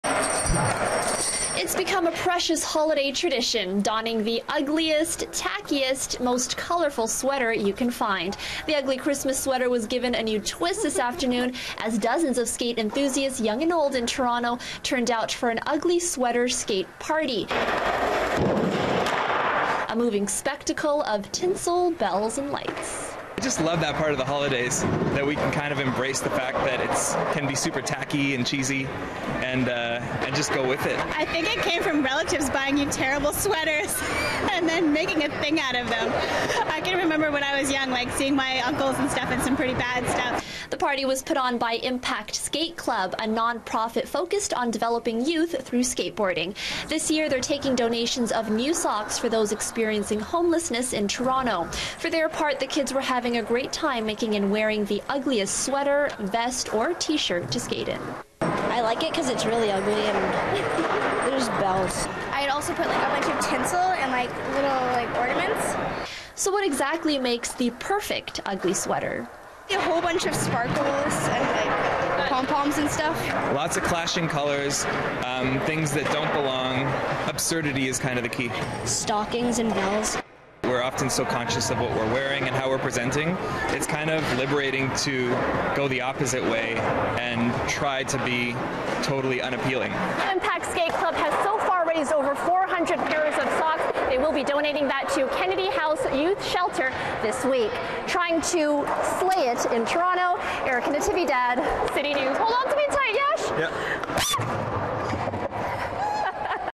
Skaters in ugly sweaters support good cause
Dozens of Toronto skate enthusiasts participated in an ugly sweater party for a good cause organized by Impact Skate Club. Erica Natividad joined in the party with a purpose.